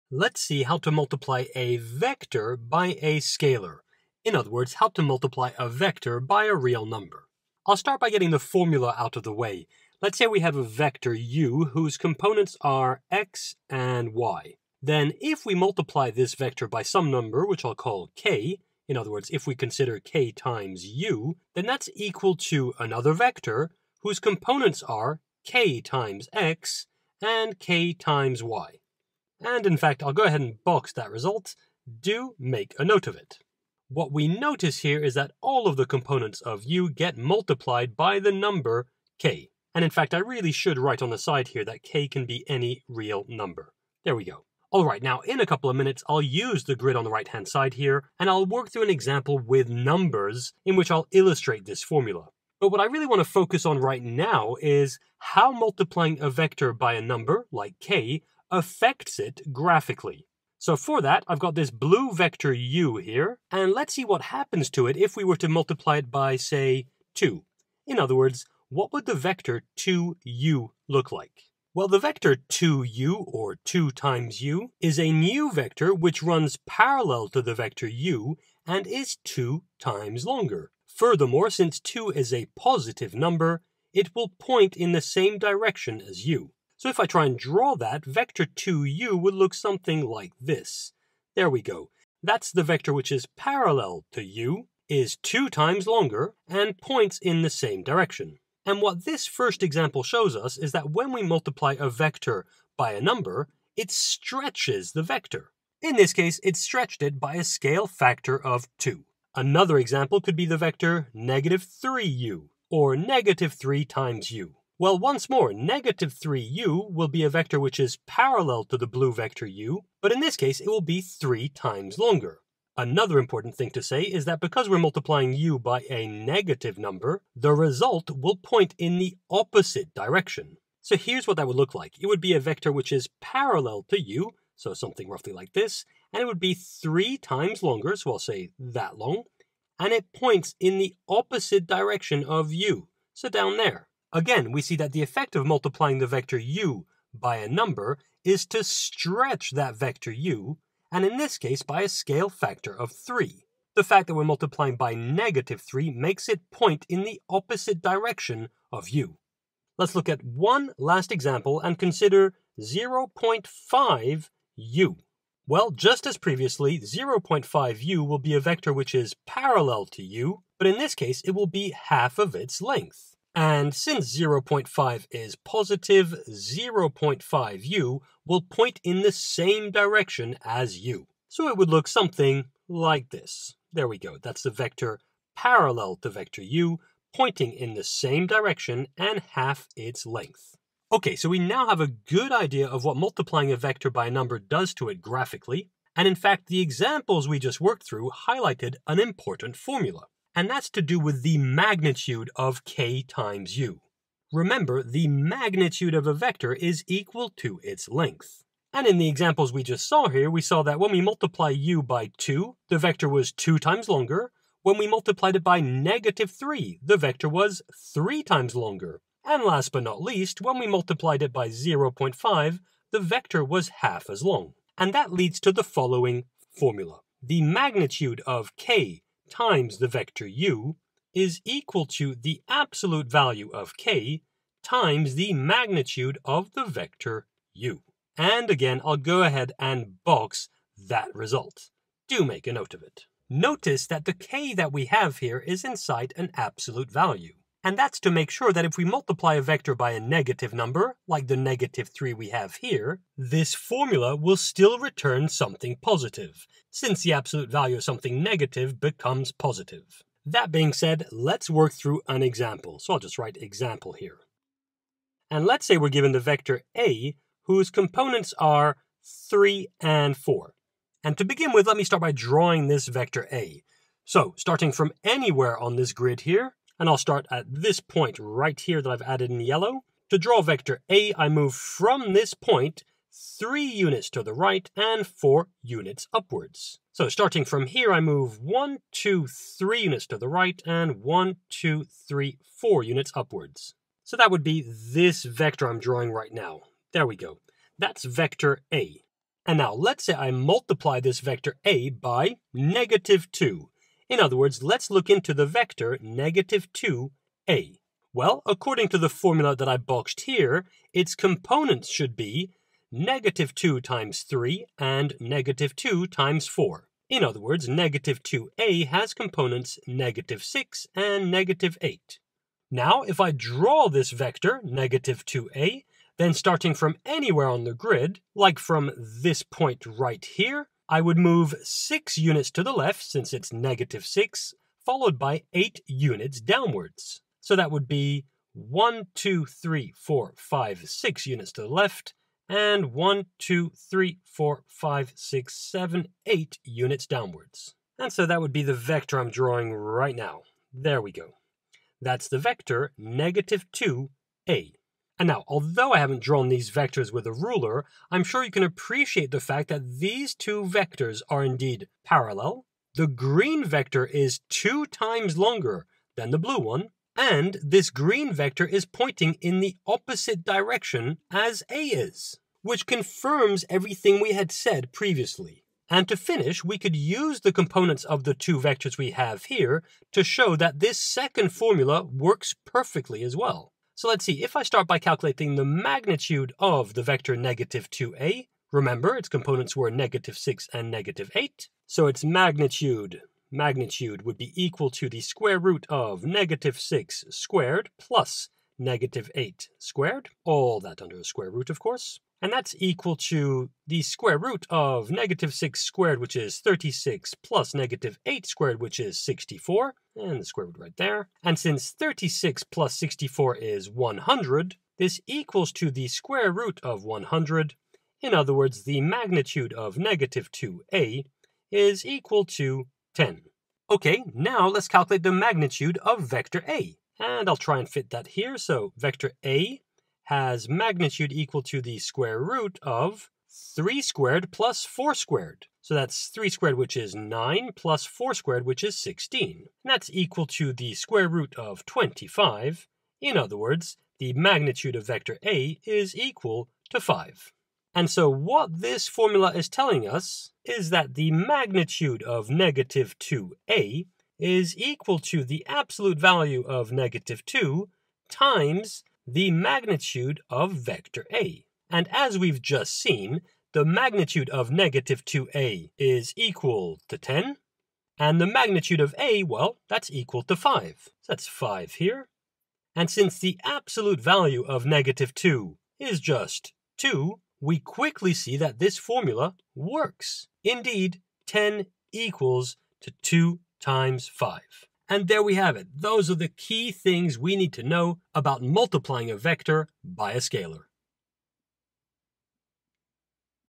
Multiplying a Vector by a Scalar - Formula & Graphical Meaning - Stretching Vectors
When multiplying a vector by a scalar k, we multiply each of its components by that scalar. The vector is then stretched by a scale factor equal to the absolute value of k and points in the same direction as the original vector if k is positive and in the opposite direction if its negative. In this tutorial we show how a vector is affected graphically and learn the formula for both : multiplication by a scalar as well as for the magnitude of a scalar multiple of a vector.